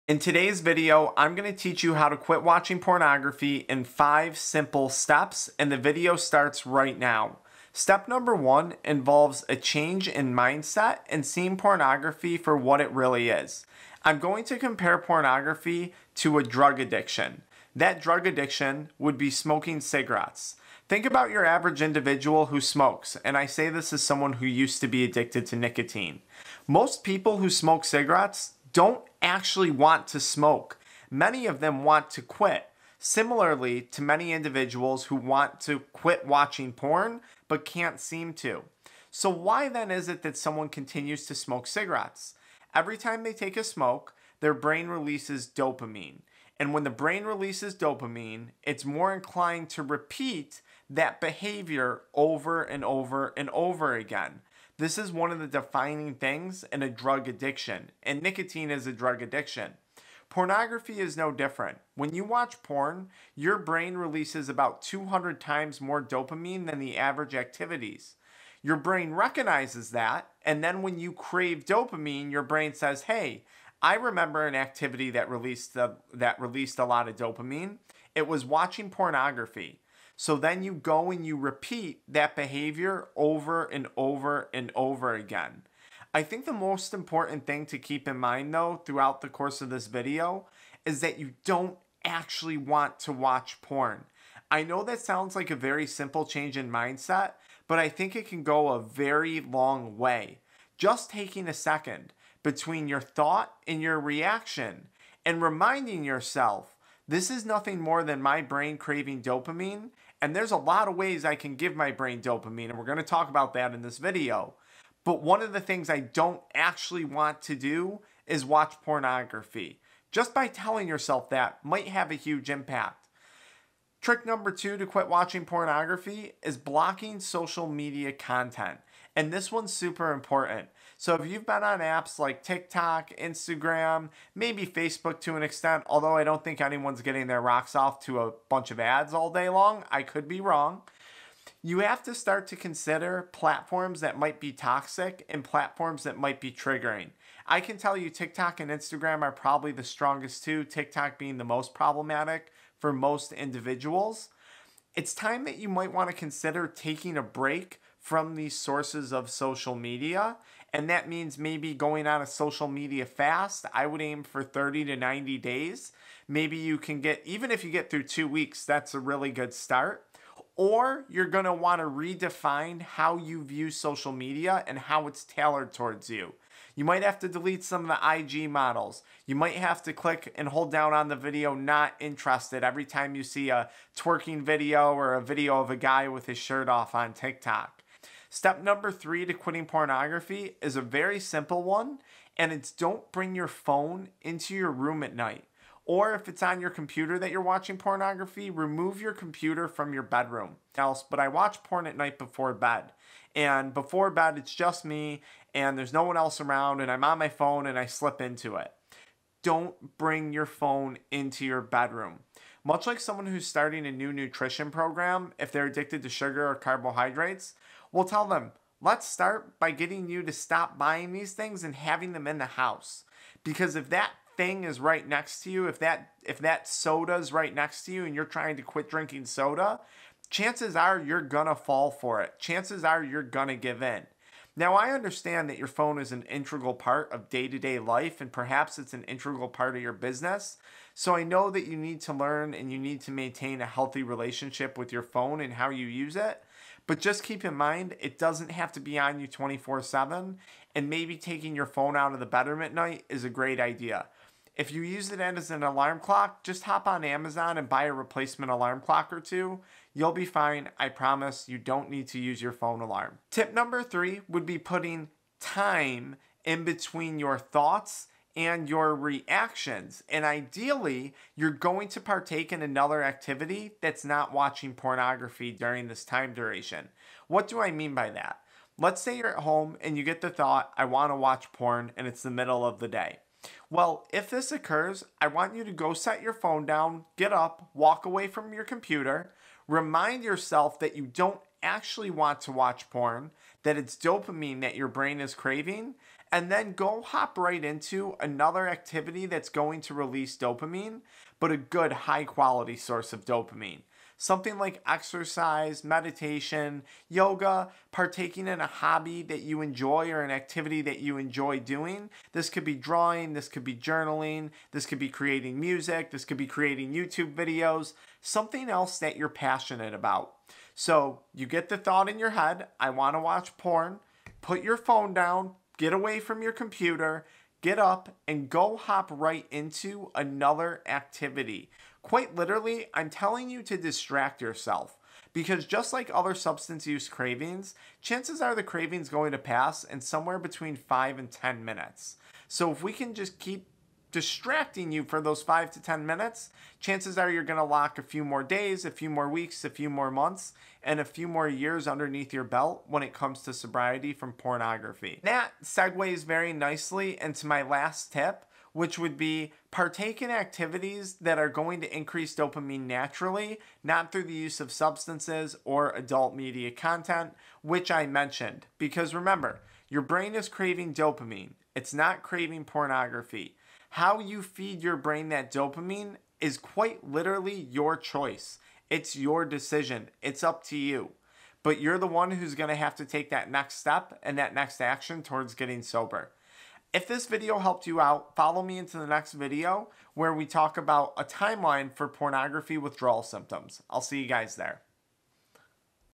How to stop PORN Addiction in five SIMPLE steps
Quitting a pornography addiction can often be a challenging task. If you are trying to quit watching porn because you are overwatching porn, this video is for you. In this brief video Dr. Frank founder of AddictionMindset Recovery coaching provides five simple steps to help you quit porn and porn hub.

Sobriety Planner to quit vaping:
Clear Lungs:
Quit Go Inhaler: Quit vaping aid
The Easy Way To Stop Smoking:

Pornography addiction is very similar to other substance use disorders. Porn addiction leads to tolerance, dependence, and withdrawal symptoms. Withdrawal from pornography is no joke, with symptoms ranging from 12-90 days in duration. It take the brain about 365 days to fully rebuild after quitting porn.

If you are struggling to quit watching porn be sure to check out the AddictionMindset website below: